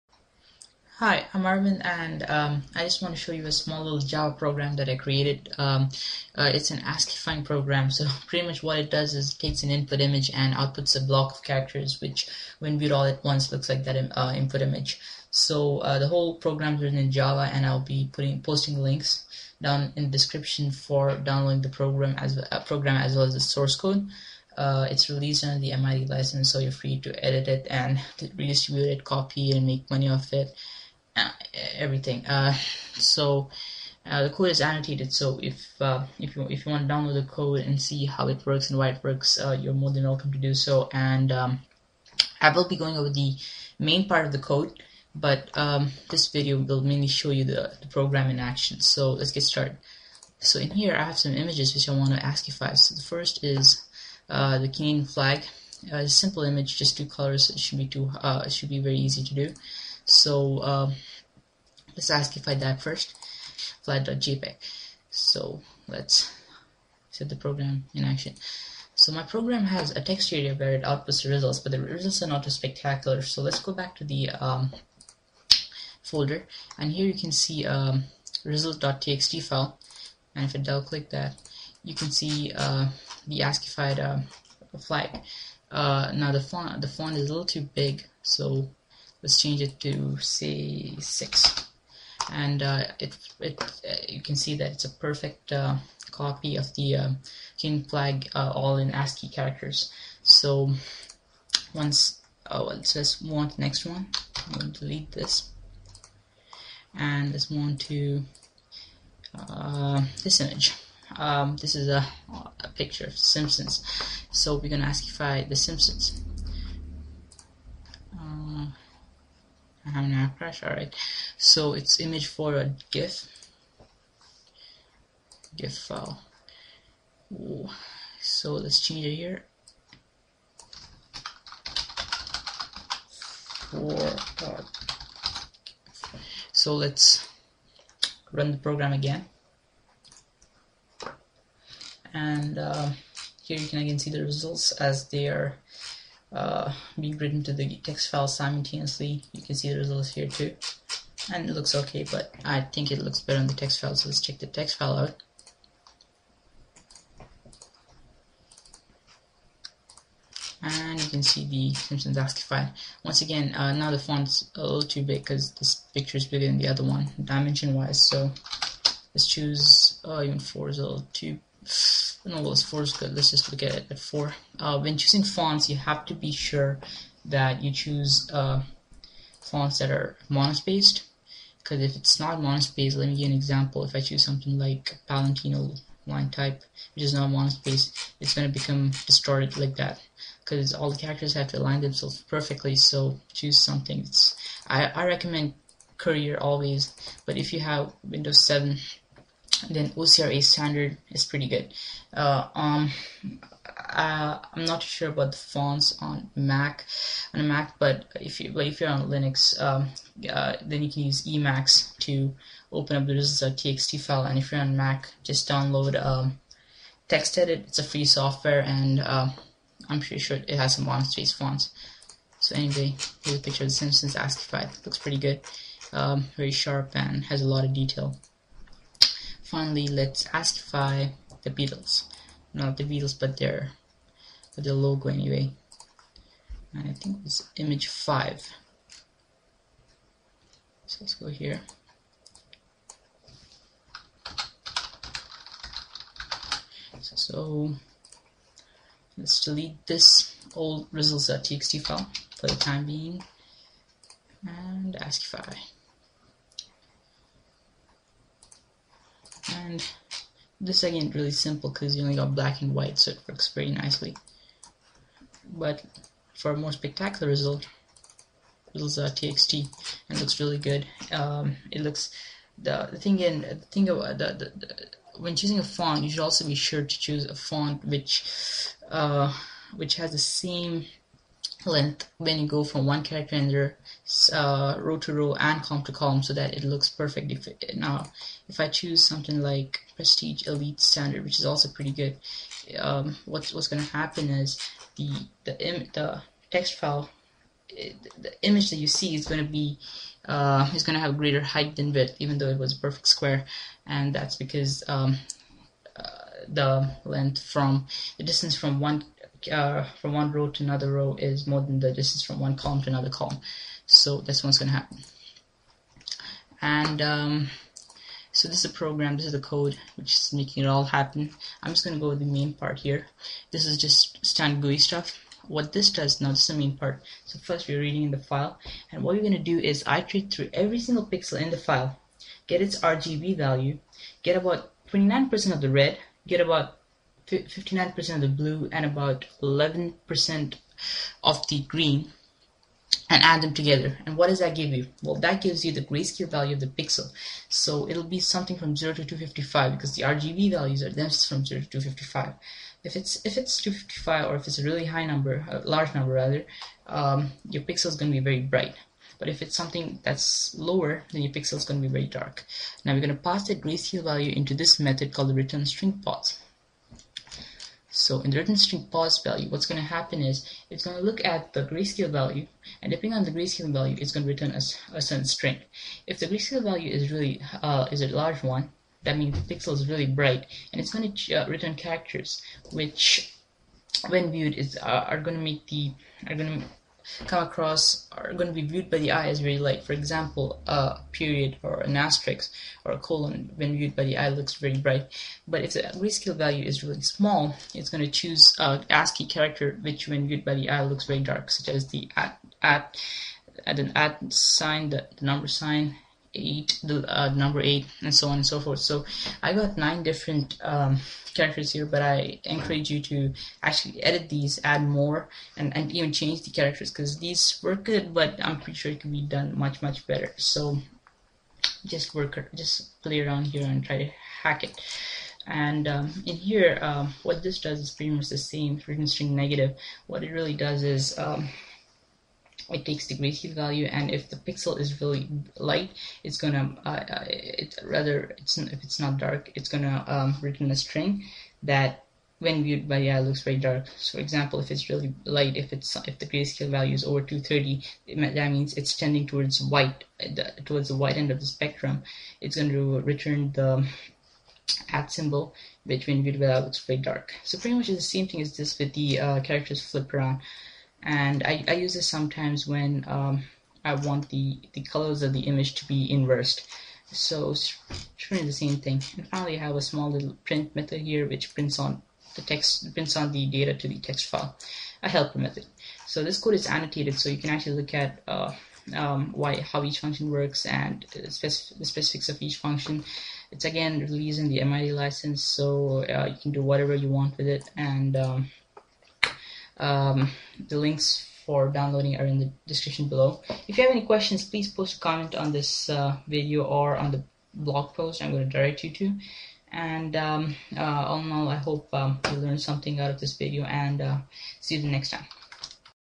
Image to ASCII Java program and source code
This is a simple Java program that takes an input image and outputs a block of ASCII characters that when looked at once looks like the input image. Source code released under the MIT license.

Link to post(you can find the tutorial, program and source code in the post):

Aravind Rao, 2011